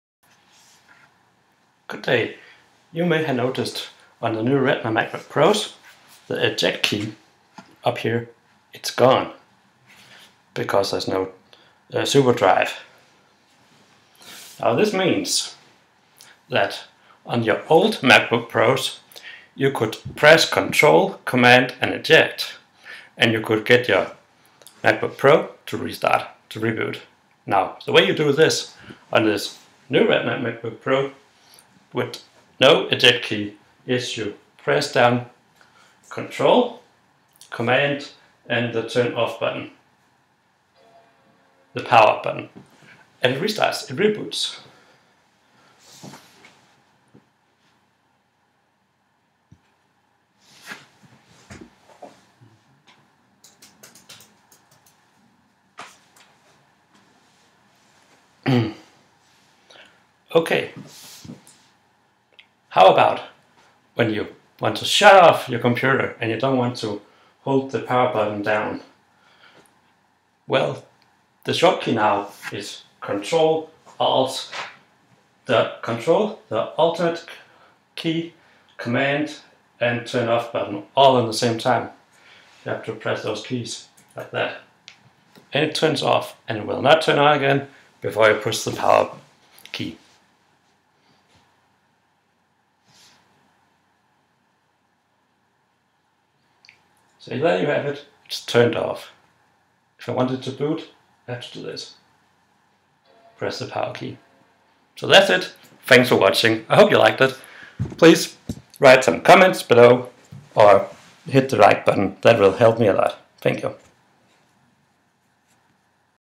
Retina MacBook Pro late 2013 short cut keys reboot and turn off
This is a how to video clip where I show how you can use short cut keys to reboot and to shut down your Retina MacBook Pro. Why ? because they chanced the keyboard slightly.